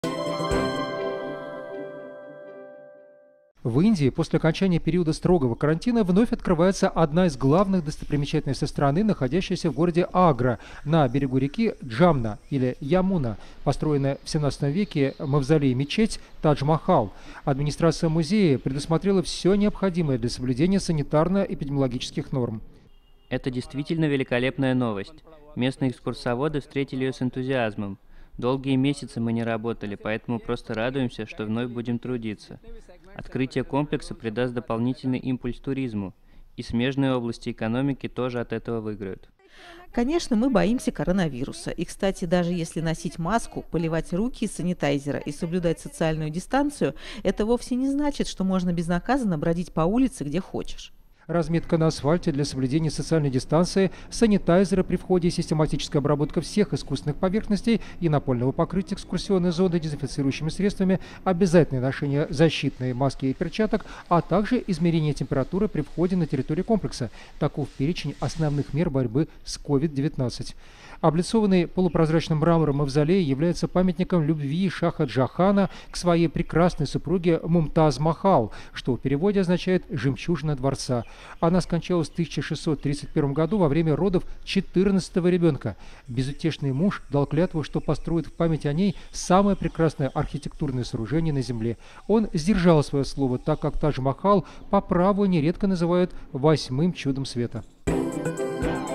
Тадж-Махал снова ждёт туристов
В Индии после окончания периода строгого карантина вновь открывается одна из главных достопримечательностей страны, находящийся в городе Агра, на берегу реки Джамна (Ямуна) - построенная в XVII веке мавзолей-мечеть Тадж-Махал.
Администрация музея предусмотрела всё необходимое для соблюдения санитарно-эпидемиологических норм.
"Это действительно великолепная новость! - говорит Шамшуддин Хан, председатель Ассоциации туристических гидов. - Местные экскурсоводы встретили её с энтузиазмом. Долгие месяцы мы не работали, поэтому просто радуемся, что вновь будем трудиться. Открытие комплекса придаст дополнительный импульс туризму. И смежные области экономики тоже от этого выиграют".
"Конечно, мы боимся коронавируса, - признаётся Мегна Авастхи, жительница Нью-Дели. - И кстати, даже если носить маску, поливать руки из санитайзера и соблюдать социальную дистанцию, это вовсе не значит, что можно безнаказанно бродить по улице, где хочешь"…
Разметка на асфальте для соблюдения социальной дистанции, санитайзеры при входе, систематическая обработка всех искусственных поверхностей и напольного покрытия экскурсионной зоны дезинфицирующими средствами, обязательное ношение защитной маски и перчаток, а также измерение температуры при входе на территорию комплекса - таков перечень основных мер борьбы с COVID-19.
Облицованный полупрозрачным мрамором мавзолей является памятником любви шаха Джахана к своей прекрасной супруге Мумтаз Махал, что в переводе означает «жемчужина дворца». Она скончалась в 1631 г. во время родов 14-ого ребёнка. Безутешный муж дал клятву, что построит в память о ней самое прекрасное архитектурное сооружение на земле. Он сдержал своё слово, так как Тадж-Махал по праву нередко называют «восьмым чудом света».